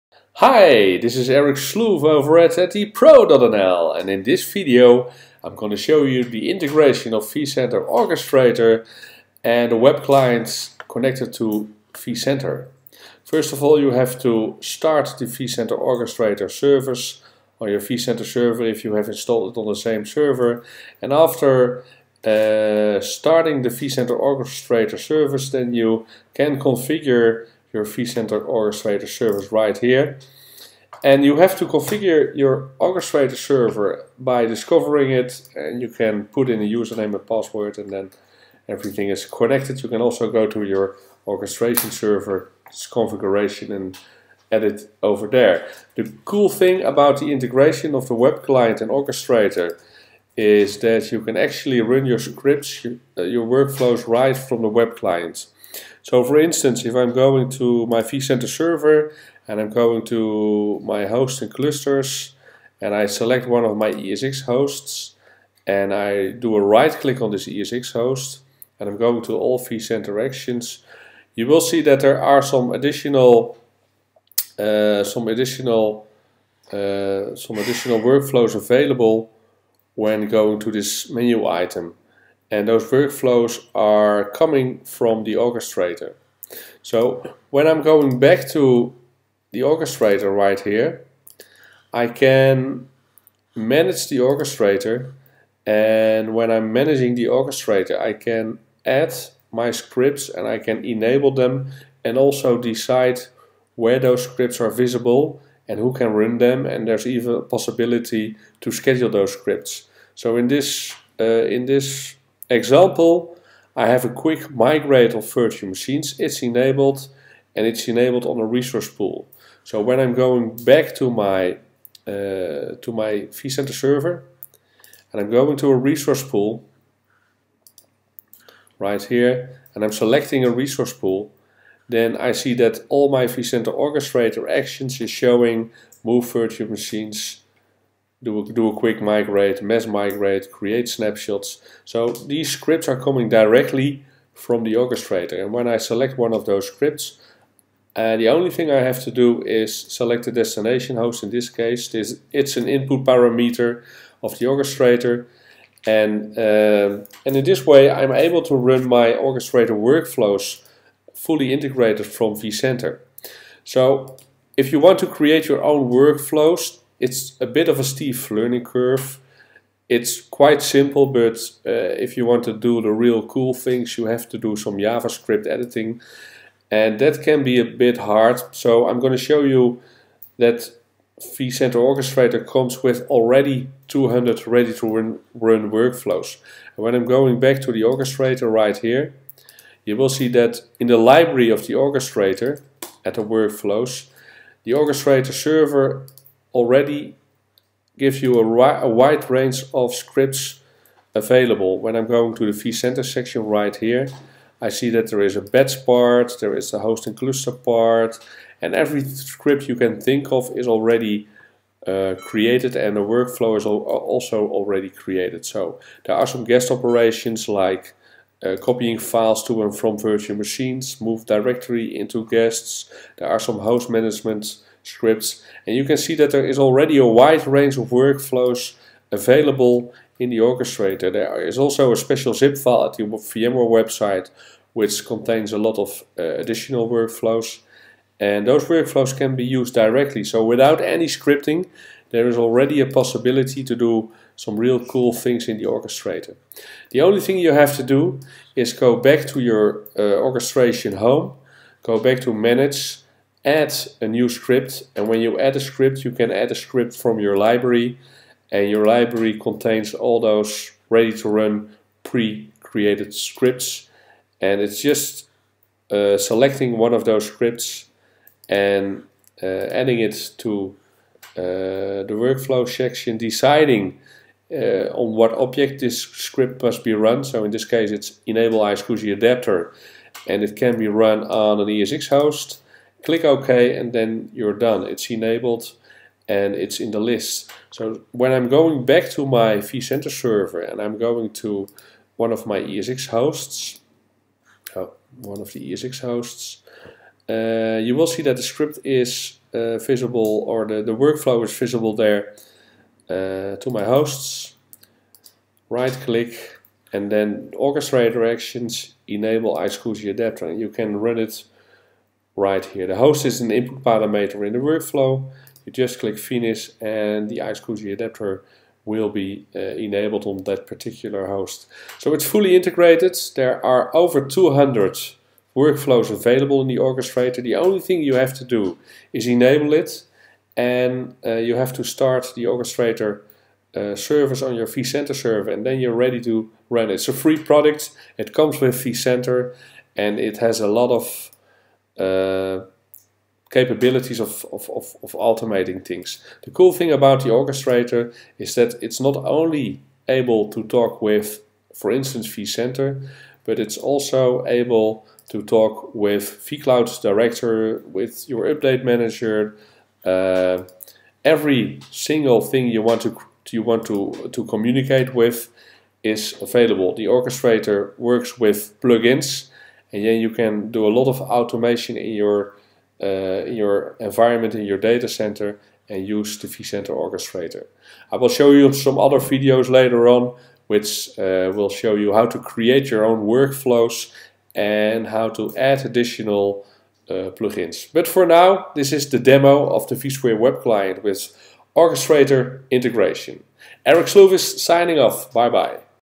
Run Workflows from the vSphere Web Client
Beyond the initial installation, operators can also take advantage of the automation capabilities of vCenter Orchestrator because of its new integration with the vSphere Web Client and the vCenter Single Sign On component. Operators can run workflows from the vSphere inventory browser in just a couple of clicks. Based on the object from which it is run, a workflow's input parameters also get populated automatically to save time and eliminate errors. Operators can run multiple workflows concurrently, or schedule them as recurring or future off-hour tasks. For large organizations, administrators can allow different groups of operators to have access to different categories of workflows.